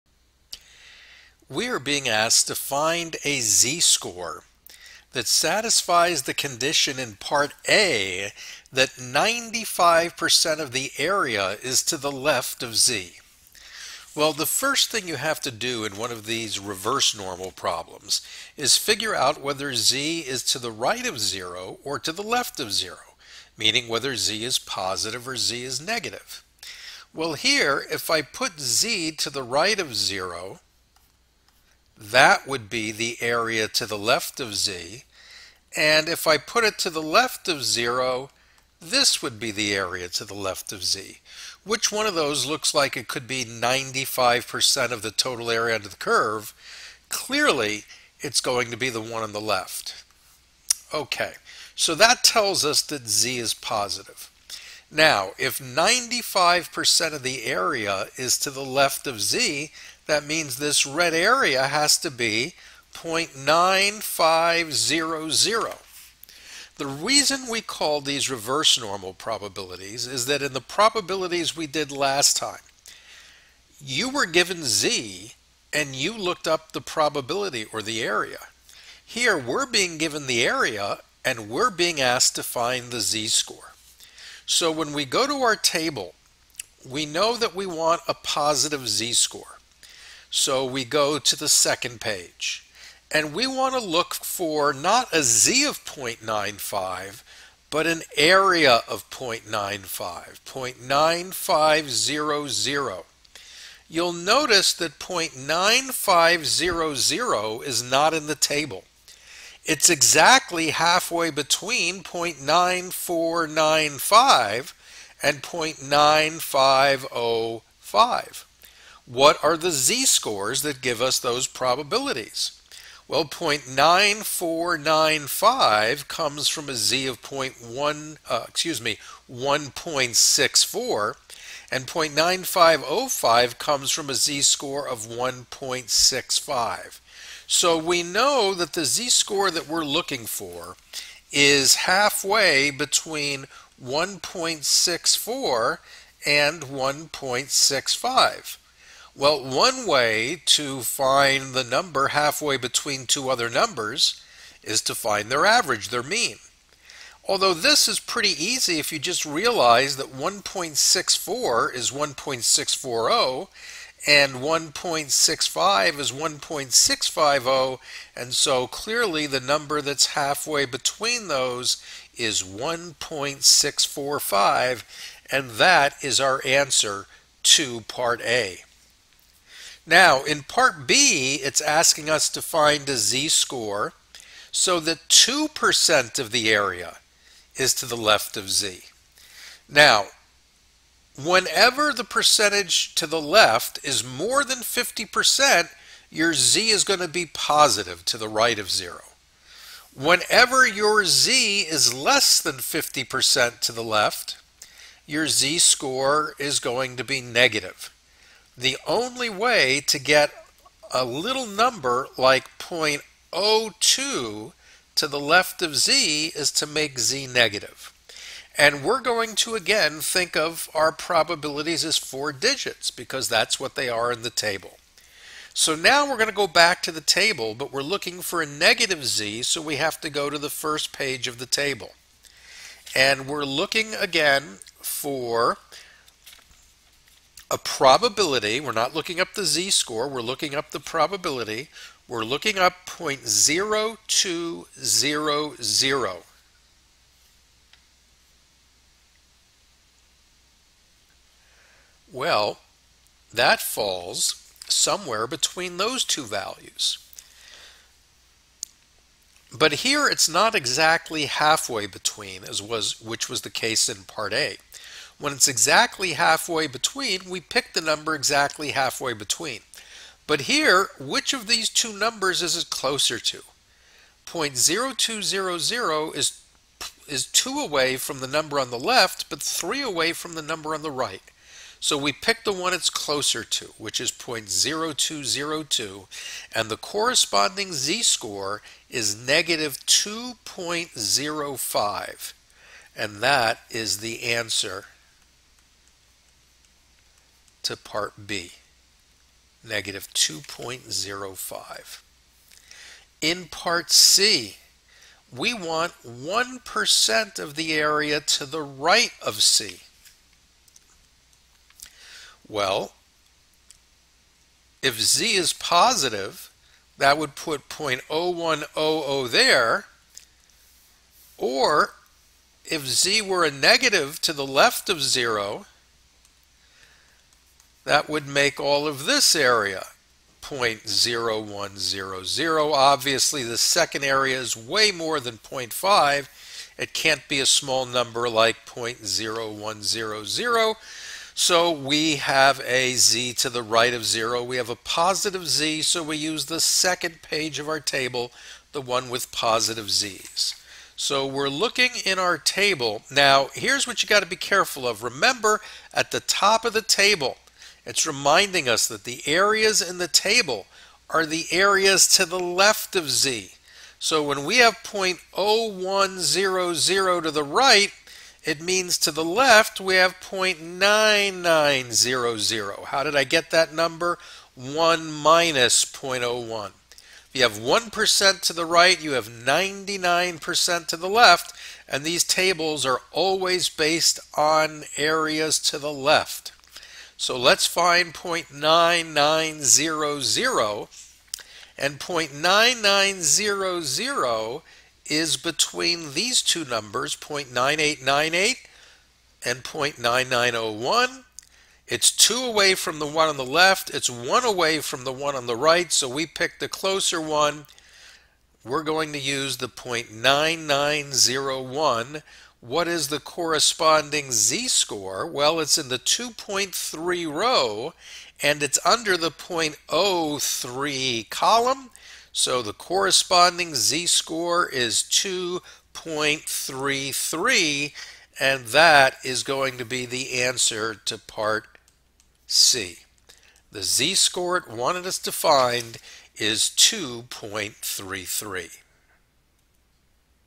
Finding z-scores given a probability
Here we reverse the procedure we learned in our last lesson and learn how to find z-scores given a probability.